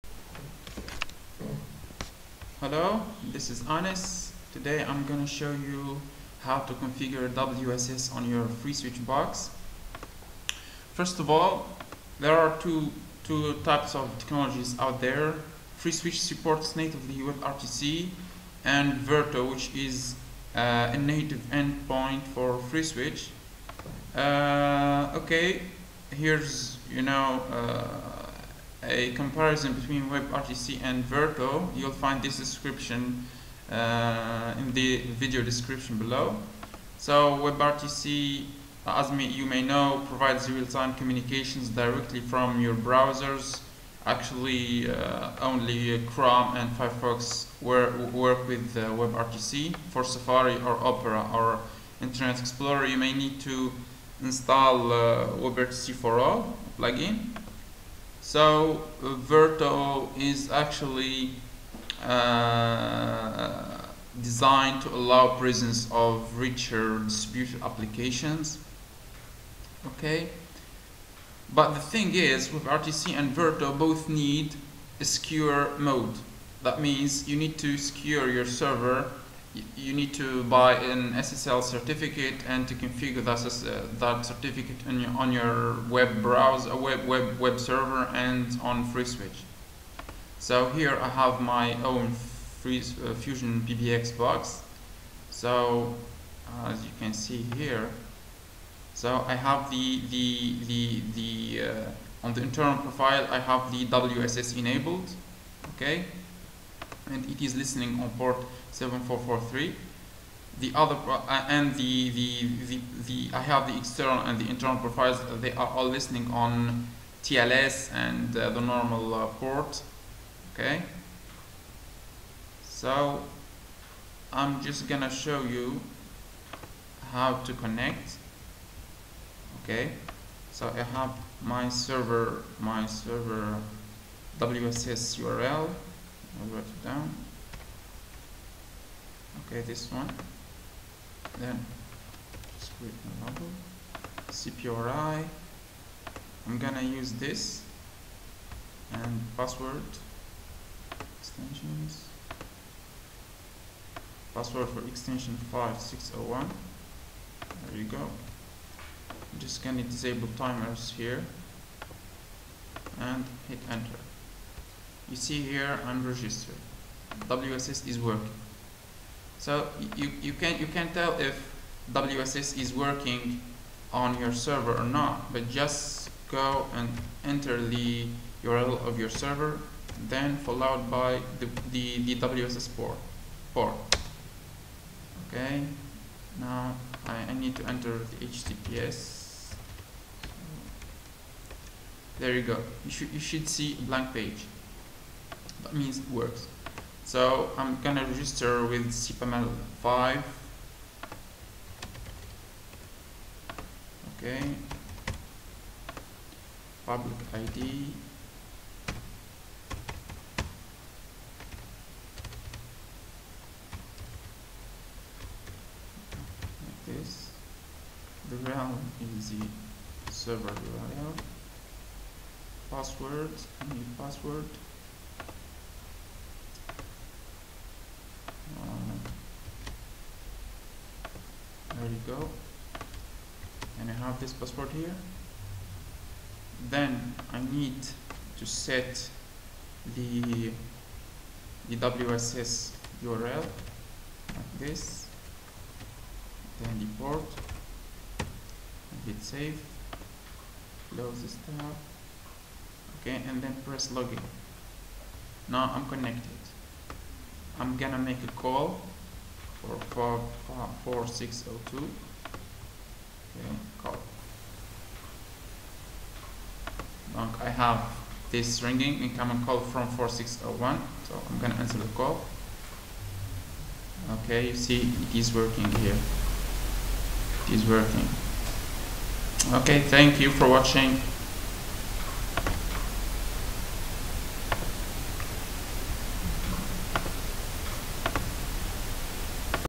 WSS FreeSWITCH
WebRTC vs Verto

WebRTC provides Real-Time Communications directly from better web browsers and devices without requiring plug-ins such as Adobe Flash nor Silverlight. WebRTC always operates in secure mode.

FreeSWITCH is both a WebRTC gateway and a WebRTC application server. It throws in the signaling plane too, with Verto. Let's introduce these concepts/functions:
FreeSWITCH is a WebRTC gateway because it's able to accept encrypted media from browsers, convert it, and exchange it with other communication networks that use different codecs and encryptions, for example, PSTN, mobile carriers, legacy systems, and others. FreeSWITCH can be a gateway between your SIP network and applications and billions of browsers on desktops, tablets, and smartphones.
FreeSWITCH is a WebRTC application server because it's able to directly provide native services to browsers, such as video conferences, IVRs, and call centers, without the use of any gateway or third-party. FreeSWITCH can directly provide services through Secure WebSocket (WSS), SRTP, and DTLS, the native WebRTC protocols.
FreeSWITCH throws in the Signaling Plane because, with Verto, browsers can initiate or receive a voice call or video call in the easiest way, and they can chat, share the screen, and receive and send data in real time to backend applications. Verto is an alternative to XMPP and SIP in JavaScript. FreeSWITCH can serve in parallel and concurrently the same application to clients that use signaling in SIP and Verto.

WebRTC is all about security and encryption. These are not an afterthought. They're intimately interwoven at the design level and are mandatory. For example, you cannot stream audio or video clearly (without encryption) via WebRTC.

Verto
Verto (VER-to) RTC is a FreeSWITCH endpoint that implements a subset of a JSON-RPC connection designed for use over secure websockets. The initial target is WebRTC to simplify coding and implementing calls from web browsers and devices to FreeSWITCH. This allows a web browser or other WebRTC client to originate a call using Verto into a FreeSWITCH installation and then out to the PSTN using SIP, SS7, or other supported protocol. This moves FreeSWITCH farther out onto the cutting edge of real-time communications technology while maintaining interoperability with SIP and other legacy protocols.
mod_verto is the signaling protocol. It depends on mod_rtc for secure media streaming services.
Verto is a web protocol that allows the presence of richer distributed applications, where audio, video, database, graphics, visualization, and augmented reality come through the same channel/tool set and converge in the browser for the user to interact with. It is deeply integrated with the FreeSWITCH events system, so it can access all of the data, primitives, statuses, and information that is available for a FreeSWITCH server-side application programmer. Also, it's very easy to write a FreeSWITCH module that will make Verto exchange data structures and events with external sources and sinks, from databases to legacy systems and automation control to collaboration platforms. Verto is an elegant weapon.